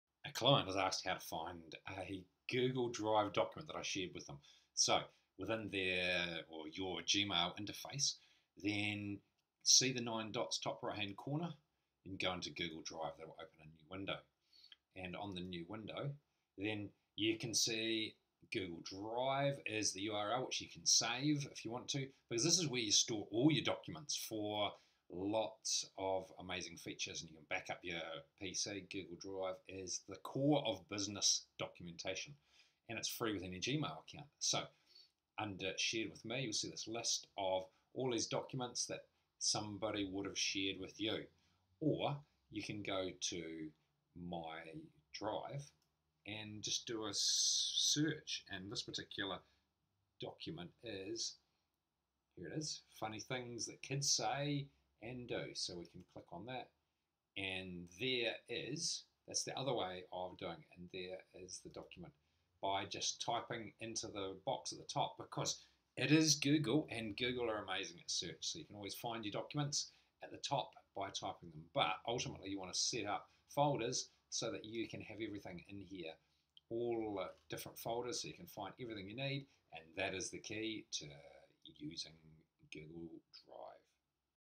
Finding shared files in G Suite Gmail Google Drive
How to navigate to Google Drive interface and Find any files. We look at finding a shared file within G Suite Gmail Google Drive, then normal files.